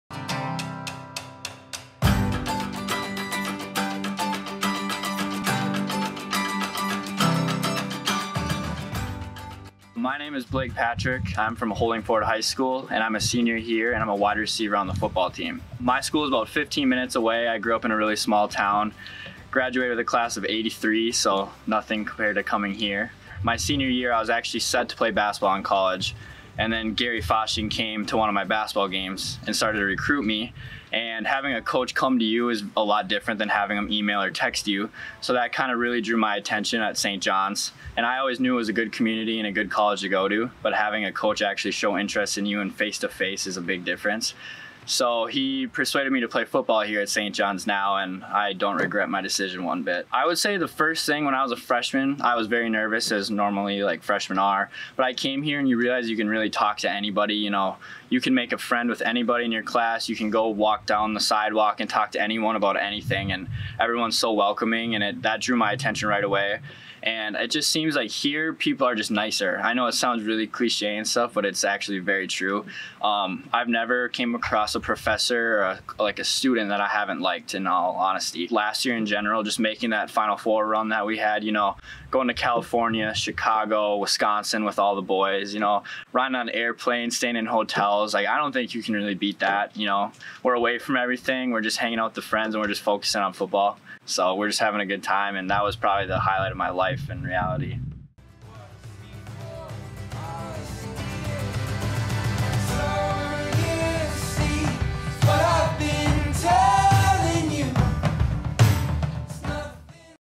Why I Chose Saint John's - Blake Patrick '21
Saint John's University (Minn.) senior wide receiver Blake Patrick talks about why he chose to attend SJU and what makes the campus community so special.